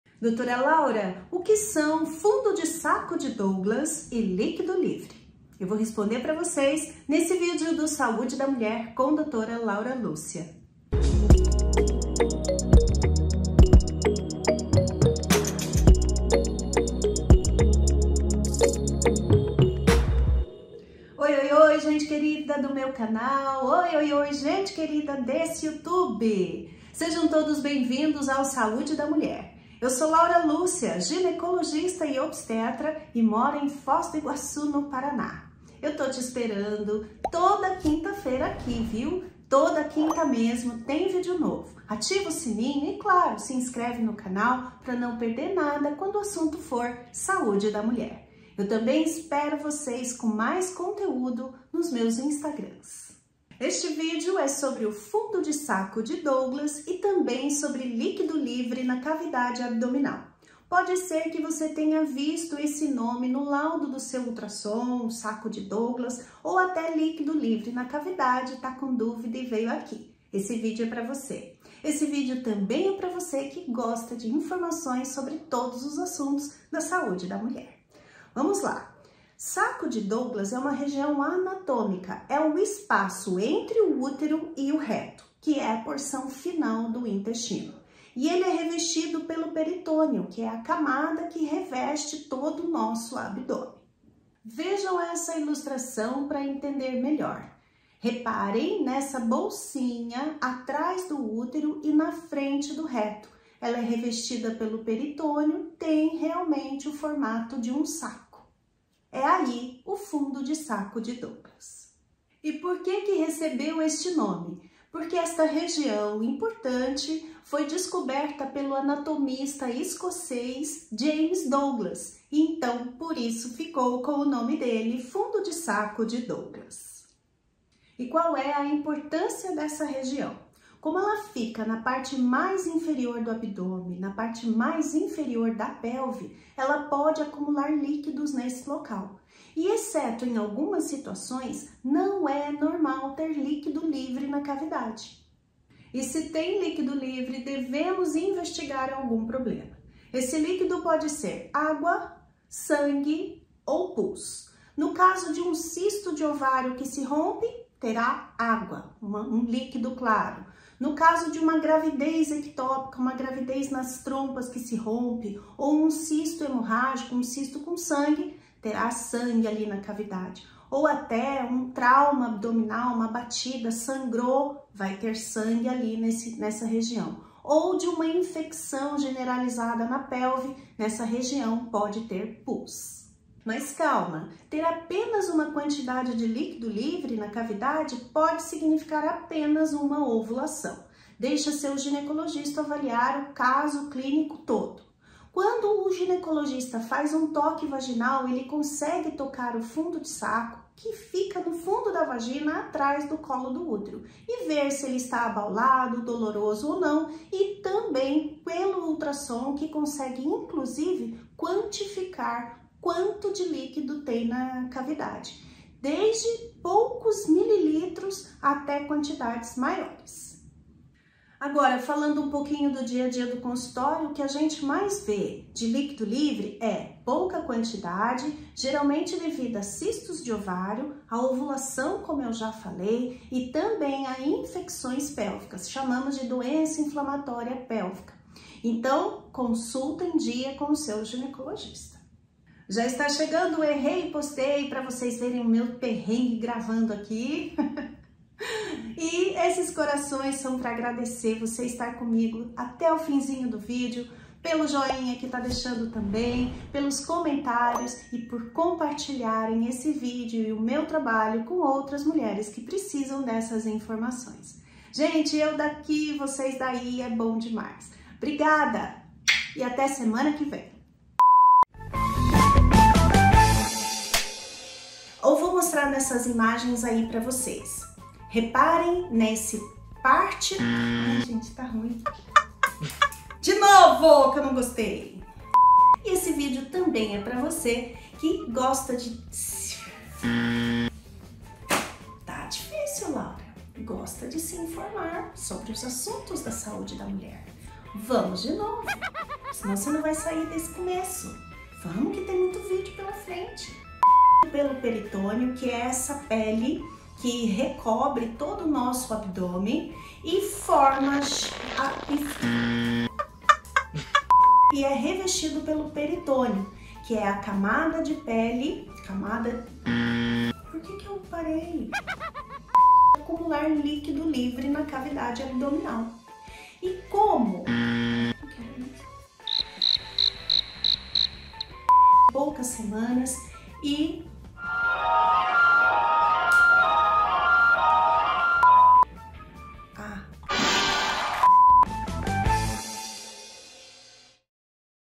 Fundo de SACO de DOUGLAS e LÍQUIDO LIVRE na cavidade.
Se o assunto é a SAÚDE DA MULHER, então pode contar comigo.
Neste vídeo explico o que são o FUNDO DE SACO DE DOUGLAS e também a presença de LÍQUIDO LIVRE na cavidade abdominal.
Obrigada por todo carinho que recebo por fazer este trabalho pra vocês.
Beijos,
Dra LL.